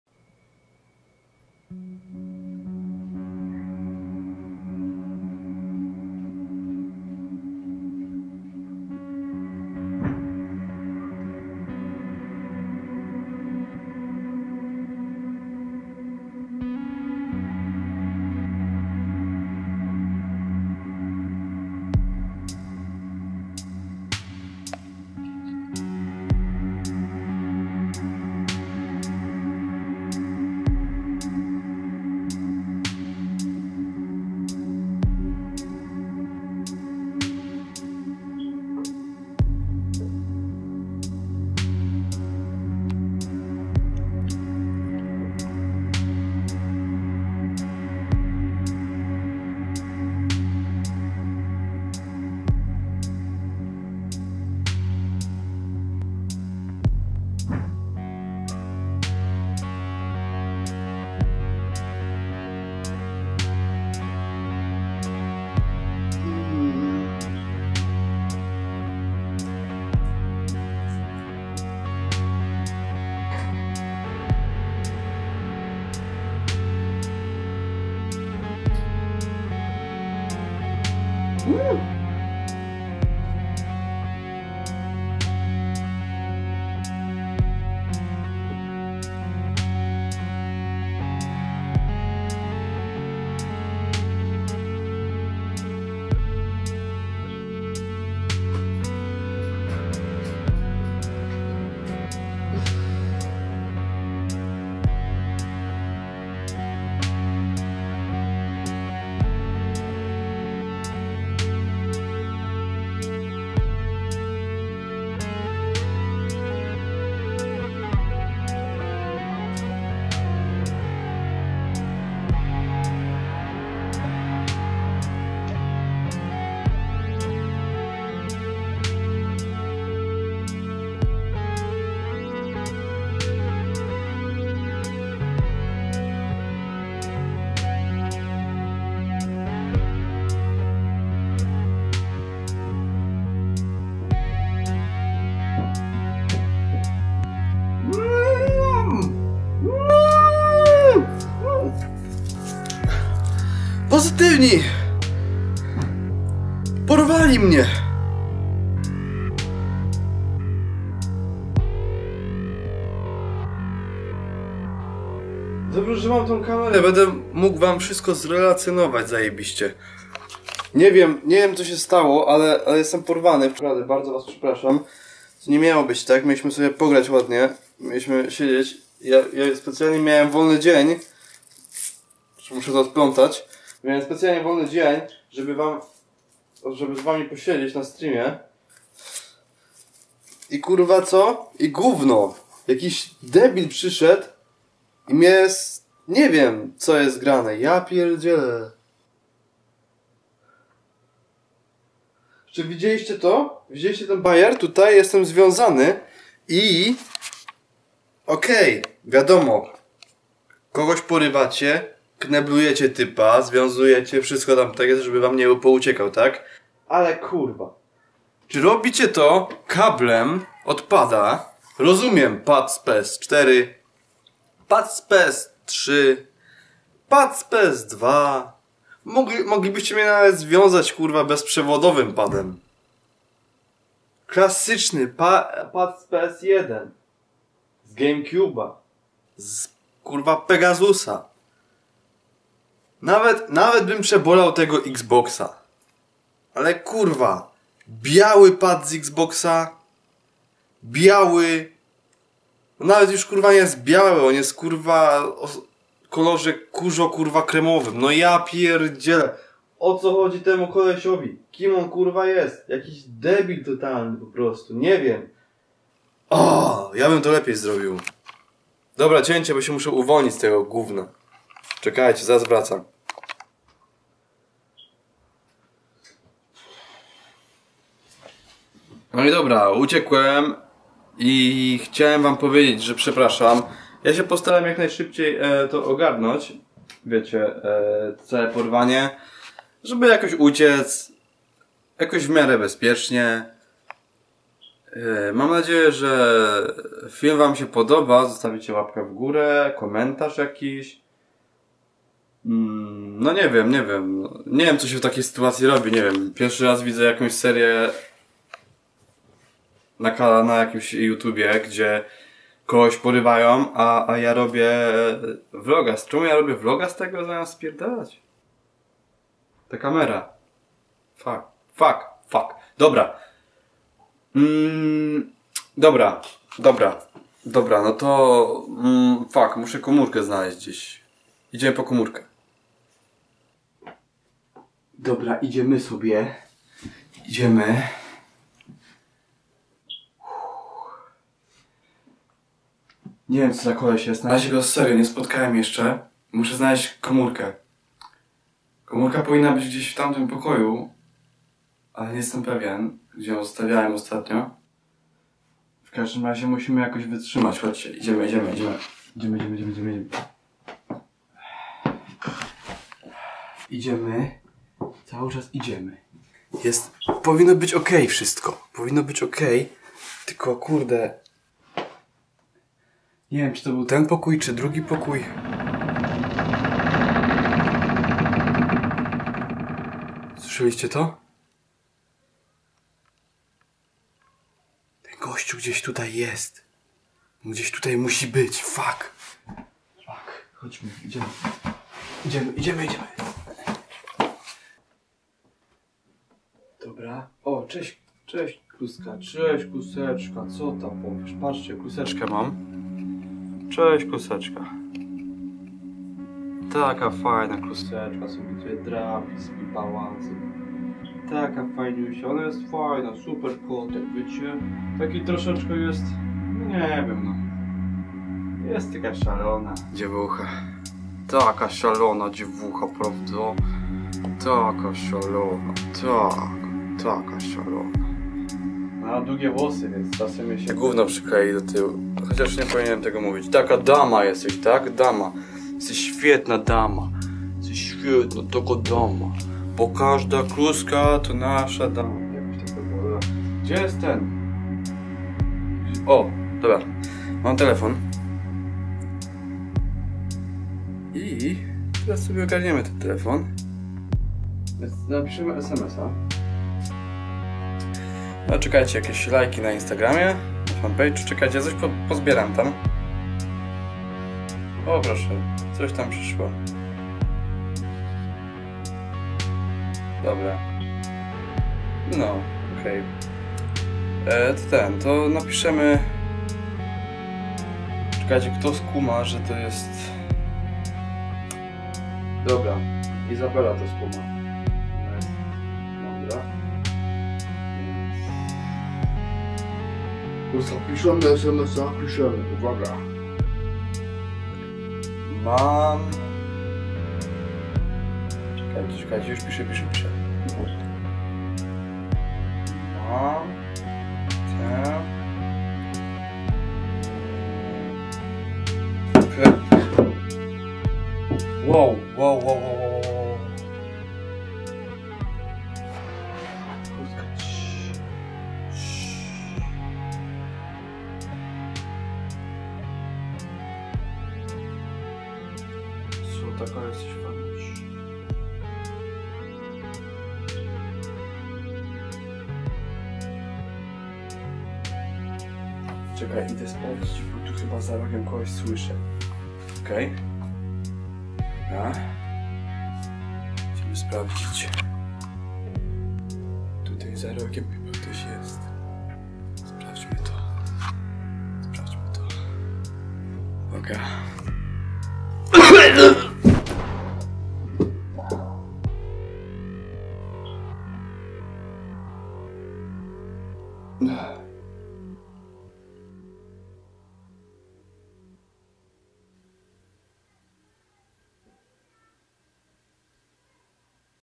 ZNANY I LUBIANY JUTUBER ZOSTAŁ PORWANY! - Prima Aprilis 2019
POZYTYWNI!
Udało mi się szybko wrzucić materiał z tej kamerki, zanim cokolwiek mi się stało...